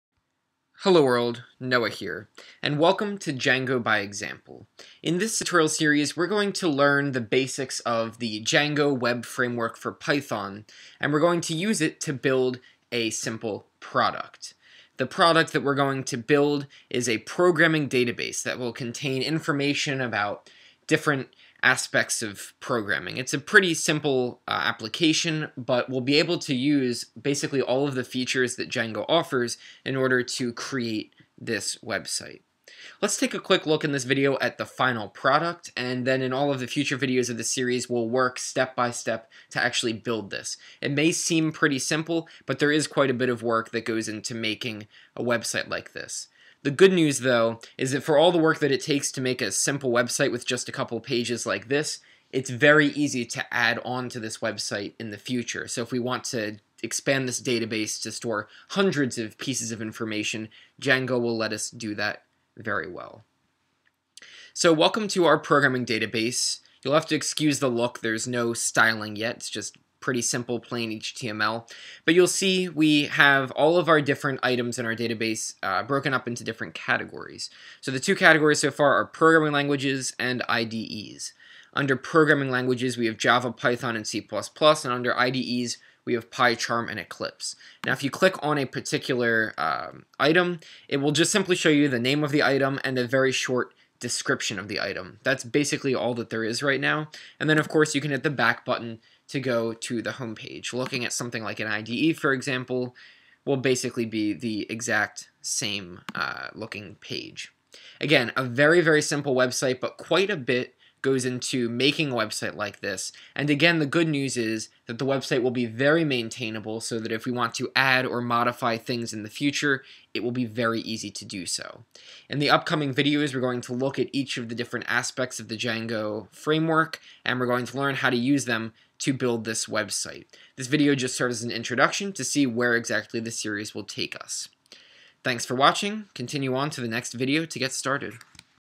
1. Intro – Django by Example
Welcome to Django by Example. In this series, we learn Django by making a simple programming database website.